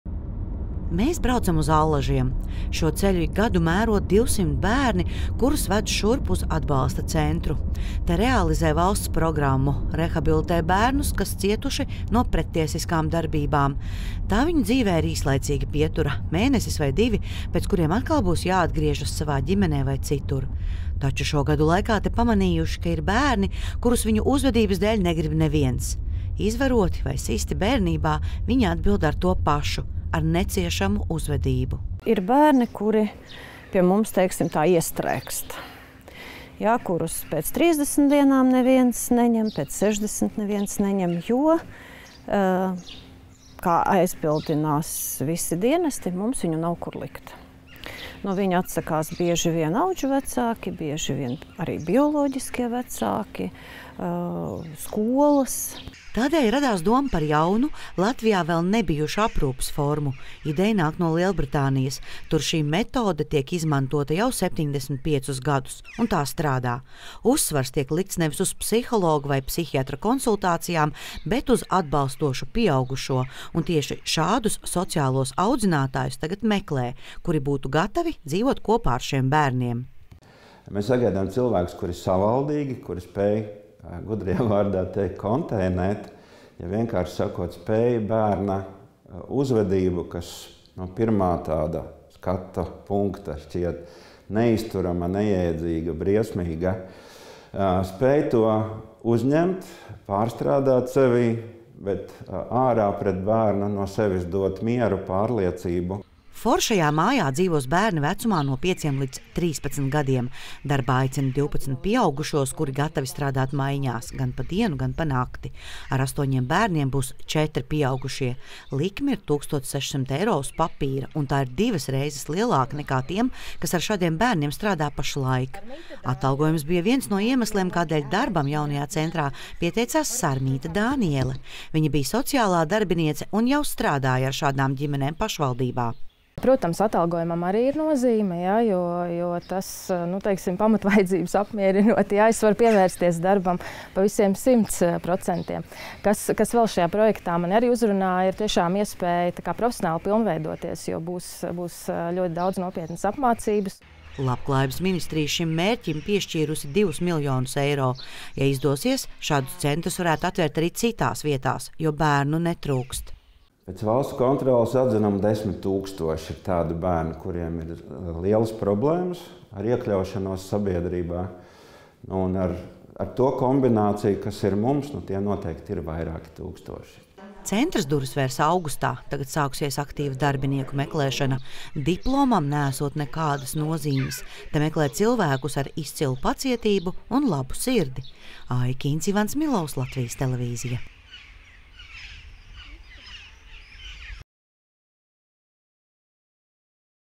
Meklē sociālos audzinātājus ar sirdi, nevis diplomu
Jau augustā Siguldas novadā durvis vērs unikāla vieta Latvijā – Foršā Māja. Tas būs aprūpes centrs bērniem, kuri pieredzējuši smagu vardarbību un kuriem valstī pieejamās atbalsta programmas nepalīdz. Viņus dēvē arī par negribētiem bērniem. Izņemti no ģimenēm, viņi mētājas pa aprūpes centriem un skolām, uzvedības dēļ no viņiem atsakās audžuģimenes. Jaunā pakalpojuma ieviesēji veidotāji tic, ka šiem bērniem var palīdzēt, šobrīd lielākā problēma – atrast audzinātājus.